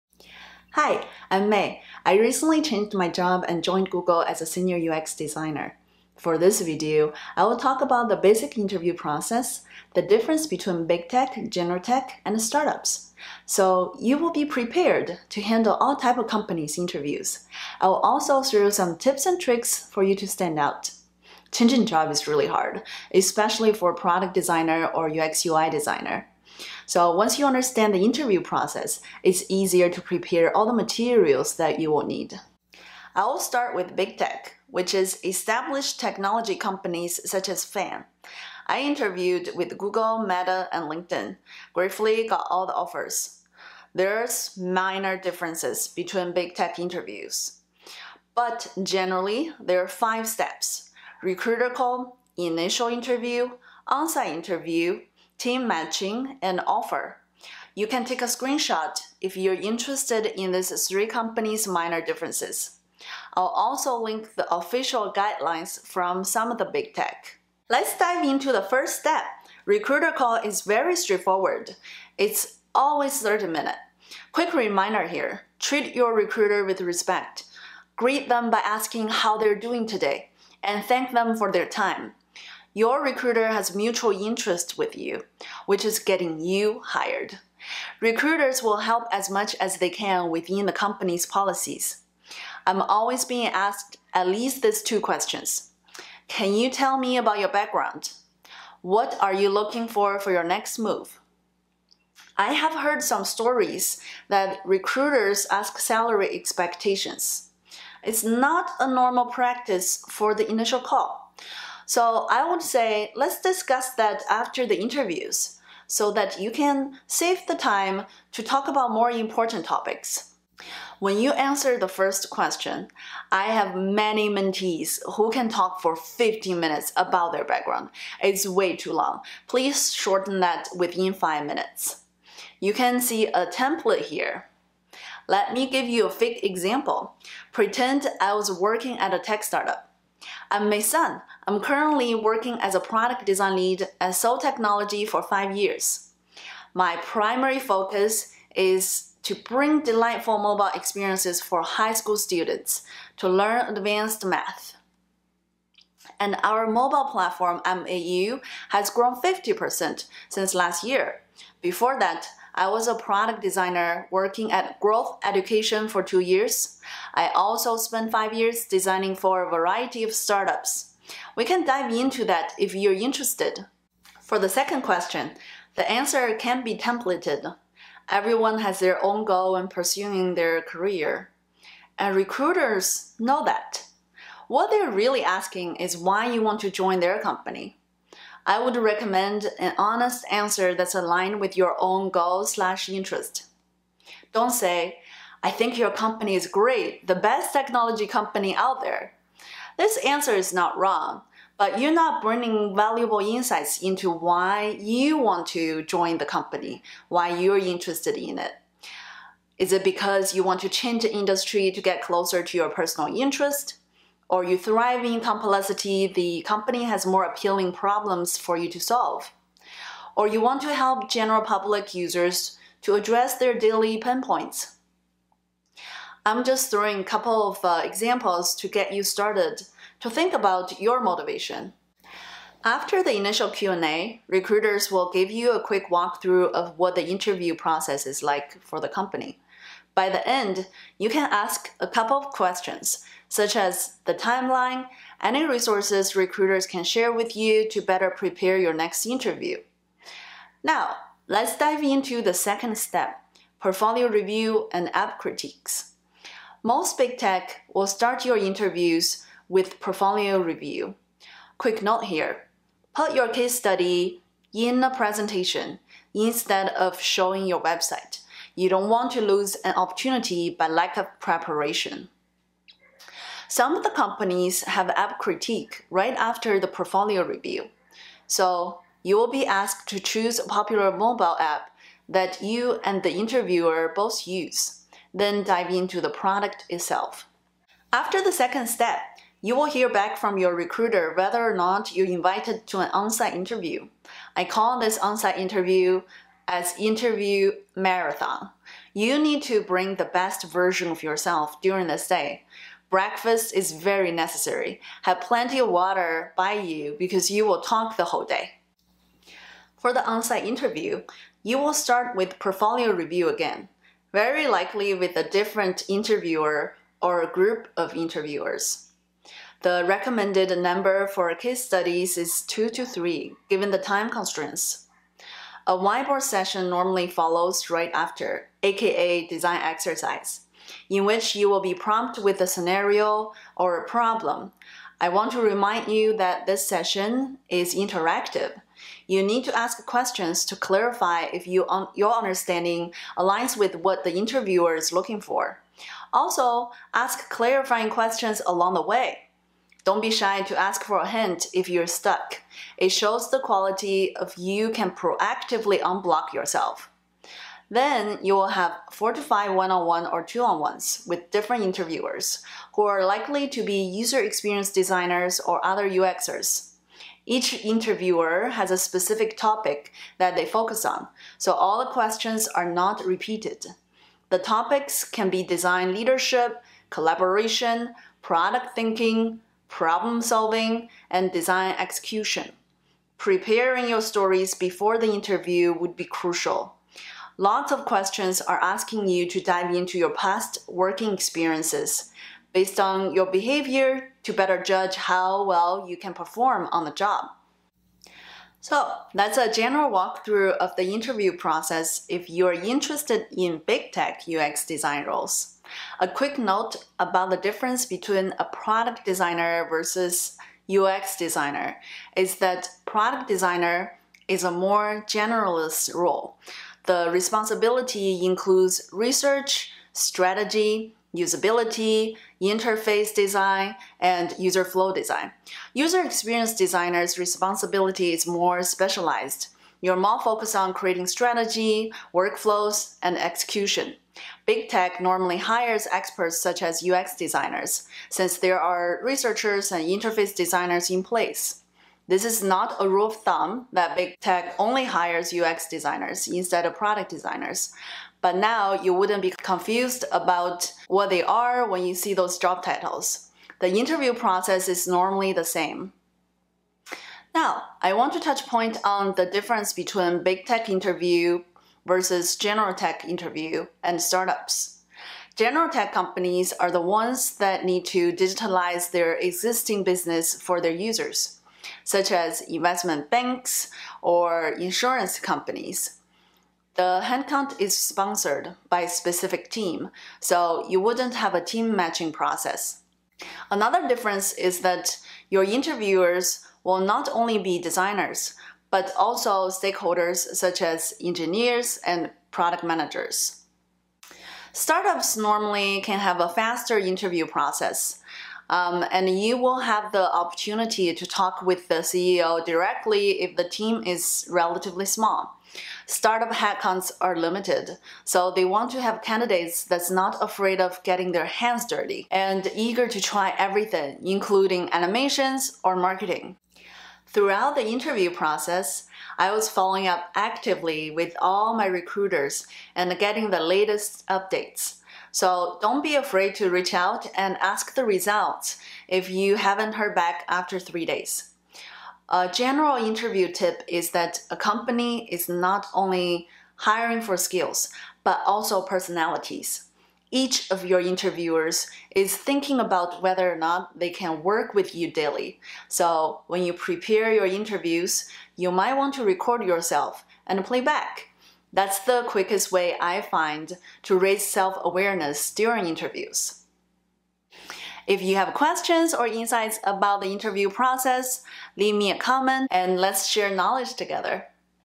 The UX/Product Design Interview Process Explained (2023) - how I landed multiple FAANG offers
In this video, I'm going to share with you my step-by-step product UX/UI design interview process guide. This guide will show you how I landed multiple FAANG offers, and how you can do the same! You will the details of  Google, Meta and Linkedin product UX/UI design interview. There are templates you can use for recruiter calls, and interview tips and tricks to standout.

You will see a breakdown of difference between product design vs UX/UI design, and how you should approach interview differently with big tech, general tech and startups.